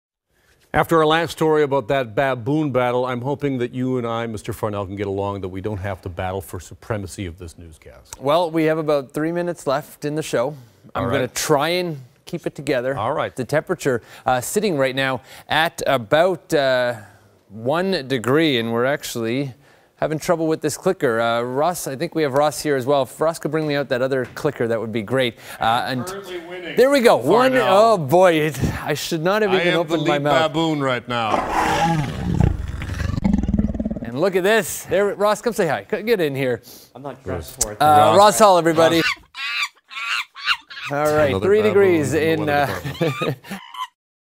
Weather Clicker Malfunction | BLOOPER (with Ross Hull)
Meteorologist Anthony Farnell experiences a clicker malfunction and fellow primate Ross Hull comes to the rescue.